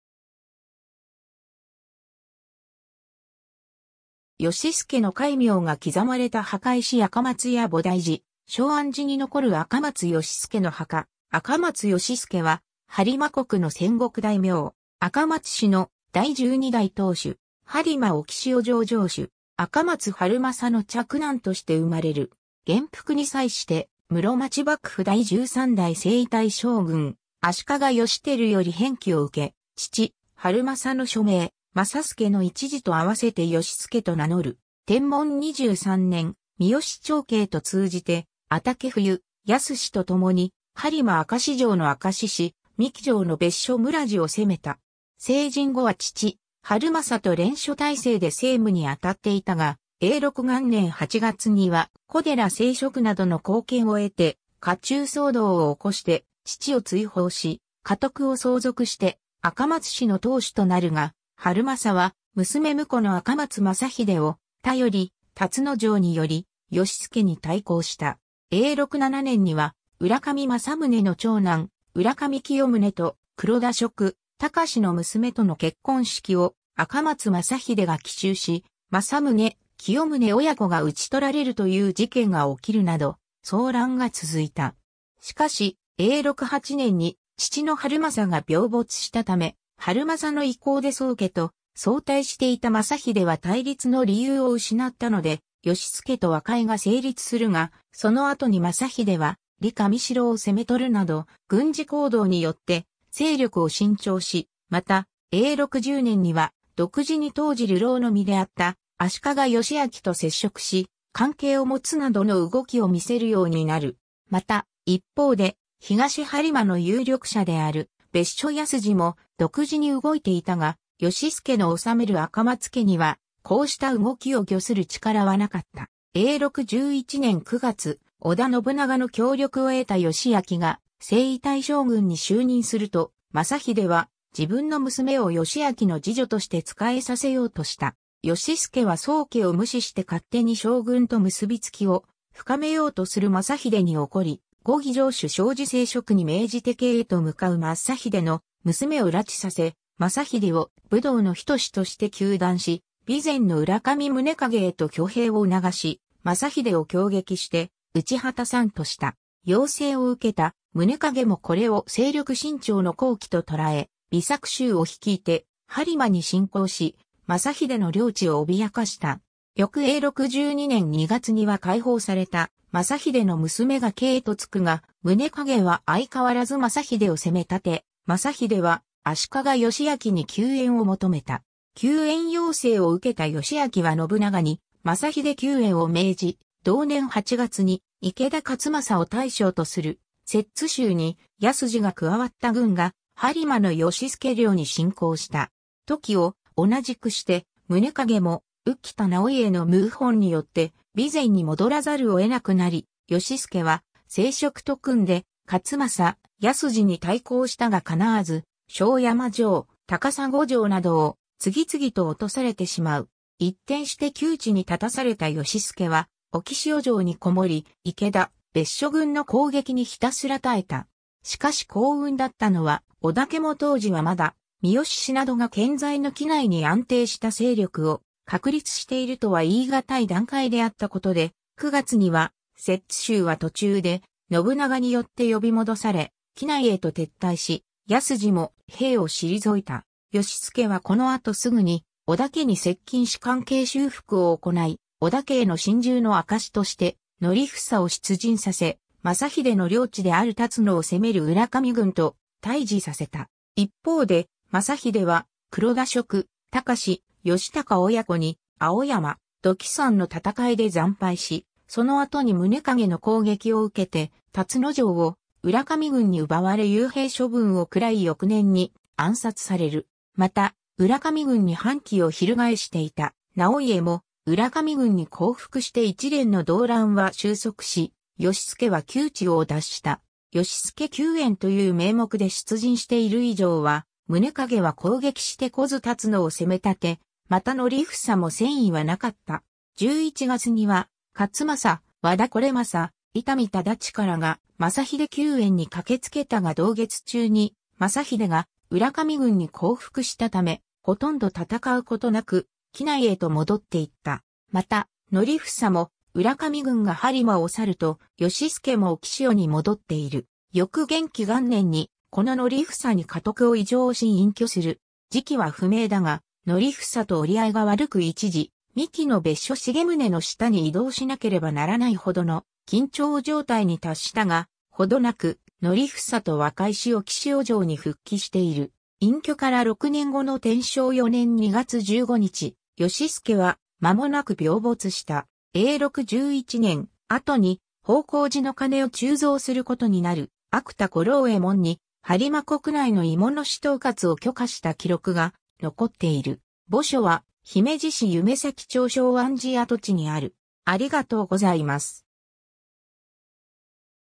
赤松義祐
義祐の戒名が刻まれた墓石 赤松家菩提寺松安寺(廃寺)に残る赤松義祐の墓 赤松 義祐(あかまつ よしすけ)は、播磨国の戦国大名。
赤松氏の第12代当主。
播磨置塩城城主。
赤松晴政の嫡男として生まれる。
元服に際して、室町幕府第13代征夷大将軍・足利義輝より偏諱（｢義｣の1字）を受け、父・晴政の初名｢政祐｣の1字と合わせて義祐と名乗る。
 天文23年（1554年）、三好長慶と通じて安宅冬康と共に播磨明石城の明石氏、三木城の別所村治を攻めた。
成人後は父・晴政と連署体制で政務にあたっていたが、永禄元年（1558年）8月には小寺政職などの後見を得て家中騒動を起こして父を追放し、家督を相続して赤松氏の当主となるが、晴政は娘婿の赤松政秀を頼り龍野城に拠り、義祐に対抗した。
永禄7年（1564年）には浦上政宗の長男、浦上清宗と黒田職隆の娘との結婚式を赤松政秀が奇襲し、政宗・清宗親子が討ち取られるという事件が起きるなど騒乱が続いた。
 しかし、永禄8年（1565年）に父の晴政が病没した為、晴政の意向で宗家と相対していた政秀は対立の理由を失ったので義祐と和解が成立するが、その後に政秀は利神城を攻め取るなど軍事行動によって勢力を伸長し、また永禄10年（1567年）には独自に当時流浪の身であった足利義昭（義輝の弟）と接触し関係を持つなどの動きを見せるようになる。
また、一方で東播磨の有力者である別所安治も独自に動いていたが、義祐の治める赤松家にはこうした動きを御する力は無かった。
 永禄11年（1568年）9月、織田信長の協力を得た義昭が征夷大将軍（室町幕府第15代将軍）に就任すると、政秀は自分の娘を義昭の侍女として仕えさせようとした。
義祐は宗家を無視して勝手に将軍と結び付きを深めようとする政秀に怒り、御着城主小寺政職に命じて京へと向かう政秀の娘を拉致させ、政秀を「無道の仁」として糾弾し、備前の浦上宗景へと挙兵を促し政秀を挟撃して討ち果たさんとした。
要請を受けた宗景もこれを勢力伸長の好機と捉え、備作衆を率いて播磨に侵攻し政秀の領地を脅かした。
翌永禄12年（1569年）2月には解放された政秀の娘が京へと着くが、宗景は相変わらず政秀を攻め立て、政秀は足利義昭に救援を求めた。
 救援要請を受けた義昭は信長に政秀救援を命じ、同年8月に池田勝正を大将とする摂津衆に安治が加わった軍が播磨の義祐領に侵攻した。
時を同じくして宗景も宇喜多直家の謀反によって備前に戻らざるを得なくなり、義祐は政職と組んで勝正・安治に対抗したが敵わず、庄山城・高砂城などを次々と落とされてしまう。
一転して窮地に立たされた義祐は置塩城に篭もり、池田・別所軍の攻撃にひたすら耐えた。
 しかし幸運だったのは、織田家も当時はまだ三好氏などが健在の畿内に安定した勢力を確立しているとは言いがたい段階であった事で、9月には摂津衆は途中で信長によって呼び戻され畿内へと撤退し、安治も兵を退いた。
義祐はこの後すぐに織田家に接近し関係修復を行い、織田家への臣従の証として則房を出陣させ、政秀の領地である龍野を攻める浦上軍と対峙させた。
 一方で、政秀は黒田職隆・孝高親子に青山・土器山の戦いで惨敗し、その後に宗景の攻撃を受けて龍野城を浦上軍に奪われ幽閉処分を食らい翌年に暗殺される。
また浦上軍に反旗を翻していた直家も浦上軍に降服して一連の動乱は終息し、義祐は窮地を脱した。
 義祐救援という名目で出陣している以上は宗景は攻撃してこず龍野を攻め立て、また則房も戦意は無かった。
11月には勝正・和田惟政・伊丹忠親らが政秀救援に駆けつけたが同月中に政秀が浦上軍に降服したため、ほとんど戦うこと無く畿内へと戻っていった。
また、則房も浦上軍が播磨を去ると義祐も置塩に戻っている。
翌元亀元年（1570年）に子の則房に家督を移譲し隠居する。
時期は不明だが、則房と折り合いが悪く一時、三木の別所重宗の下に移動しなければならない程の緊張状態に達したが、程なく則房と和解し置塩城に復帰している。
隠居から6年後の天正4年2月15日（1576年3月15日）、義祐は間も無く病没した。
 永禄11年（1568年）、後に方広寺の鐘を鋳造することになる芥田五郎右衛門に播磨国内の鋳物師統括を許可した記録が残っている。
墓所は姫路市夢前町松安寺跡地にある。